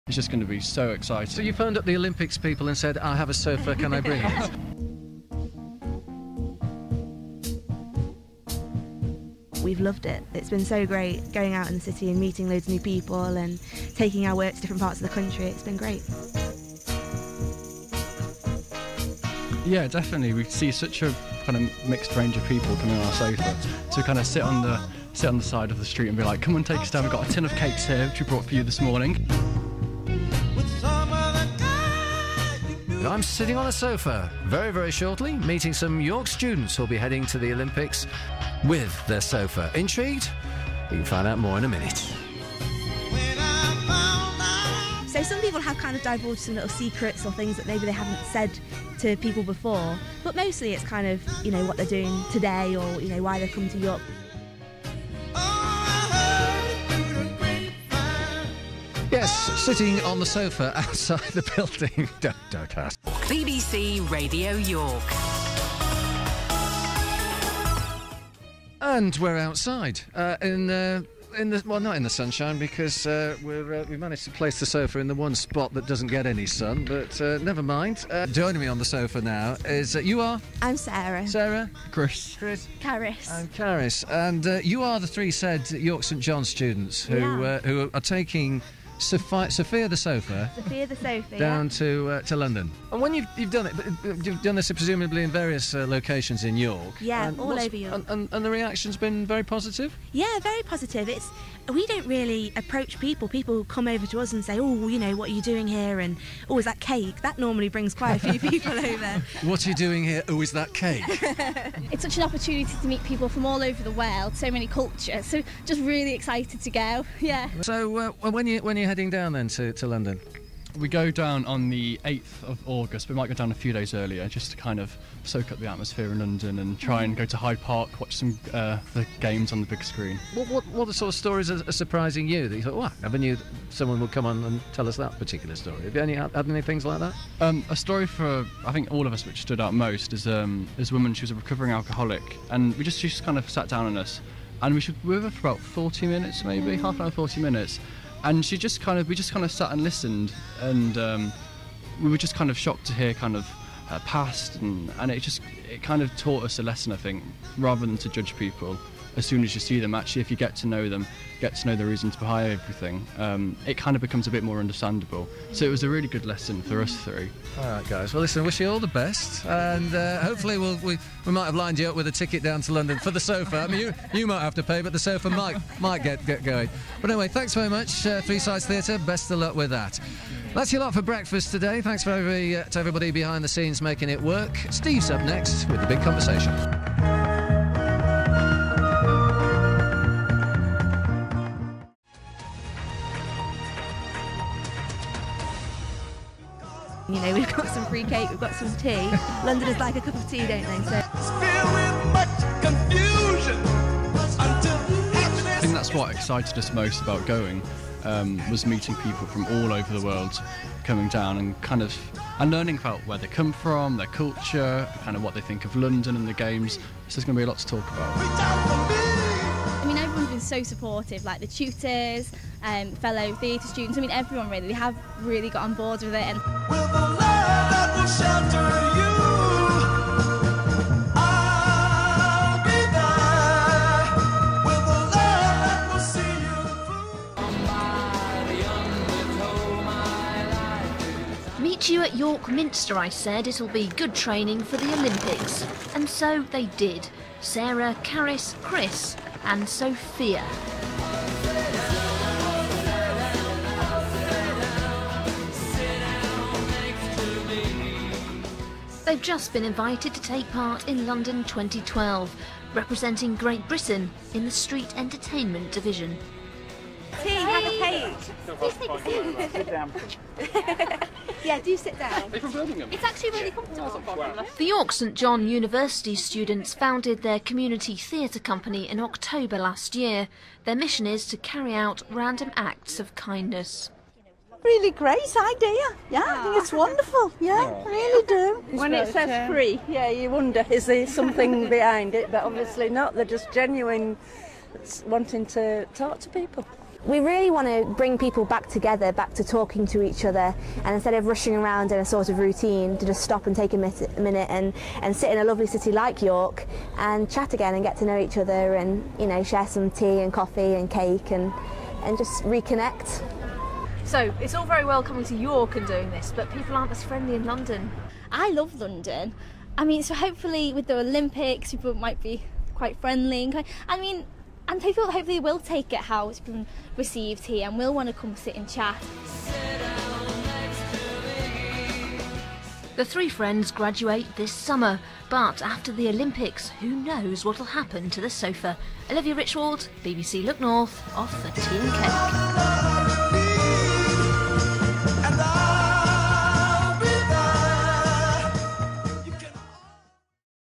Behind the Scenes of BBC Radio York/BBC Look North - Three Sides Theatre
Three Sides Theatre spent the day with BBC Radio York followed by BBC Look North as they look to head down to London and keep spirits high during the London 2012 Olympics.

[Video by Signpost Media]